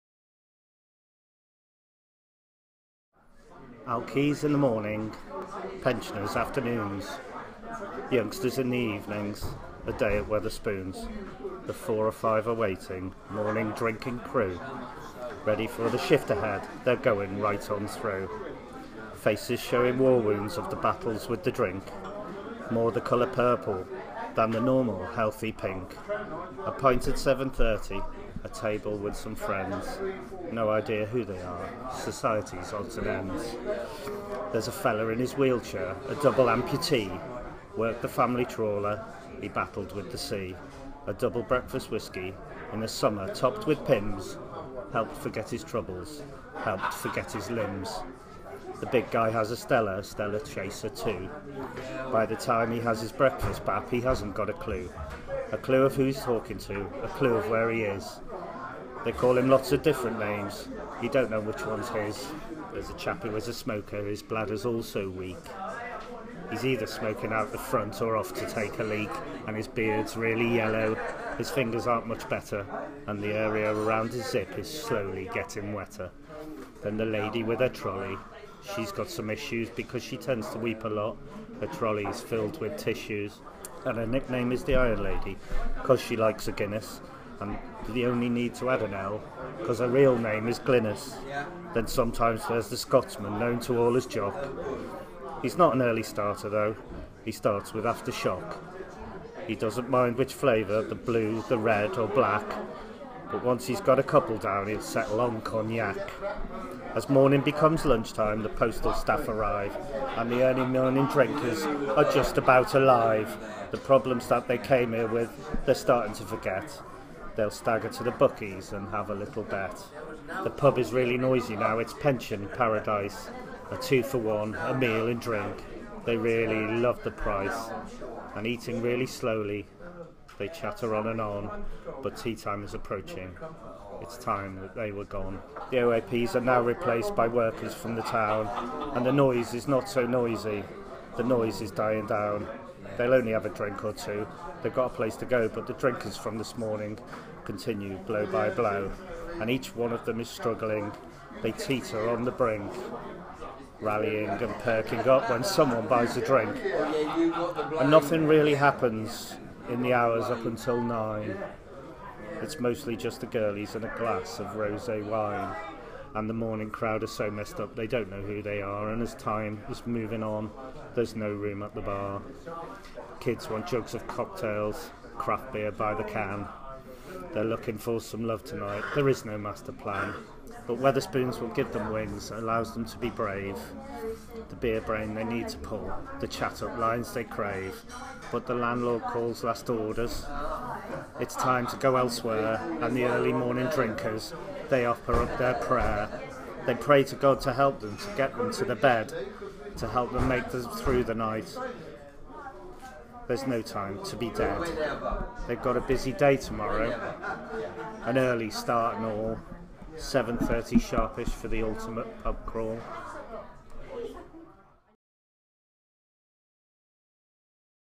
Wetherspoons: A poem by Fat Frank
A poem about the British drinking institution. Filmed on location.